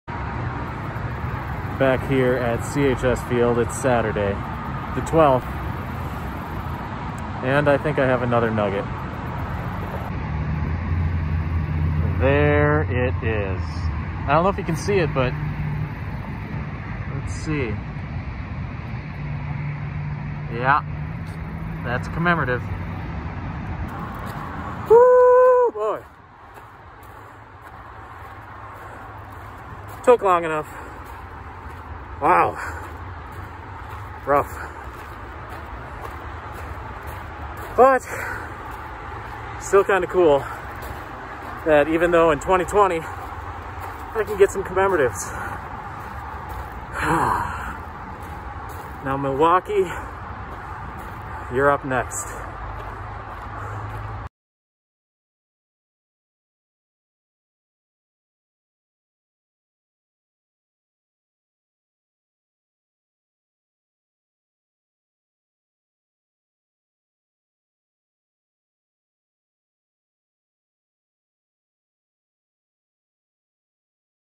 Minnesota Twins Practicing at CHS Field (Saint Paul Saints)
Well, 2020 was weird, wasn't it? I hope this is a cautionary tale to future years/generations... if you do NOT take pandemics seriously, this is the BEST case scenario.

In all honesty, we didn't deserve baseball or any of the luxury/non-essential stuff that we fought so hard for. As I am writing this (November 10, 2020) the pandemic has swelled yet again and likely to multiply our 300K+ deaths in America. I am sorry for the ways that I have fed this major incompetence.

...and start making suggestions for the charity you'd like to see supported in the comments below.